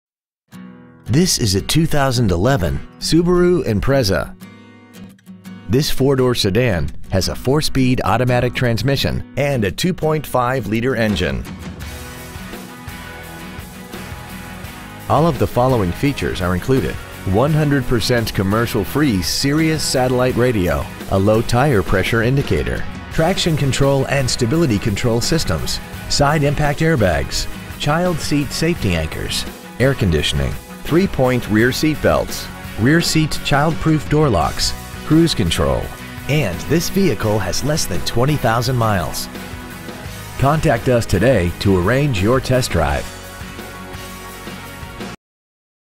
2011 SUBARU IMPREZA Sheridan WY
We have been honored to serve the Sheridan WY area , we promise that your experience at our dealership will exceed your expectations ! Year : 2011 Make : SUBARU Model : IMPREZA Engine : 2.5L H4 16V MPFI SOHC Trans . : 4-SPEED AUTOMATIC Exterior : DARK GRAY METALLIC Miles : 19,683 Interior : CARBON BLACK Stock : 4P13043 Fremont Motor Sheridan, Inc. Coffeen Avenue Sheridan , WY 82801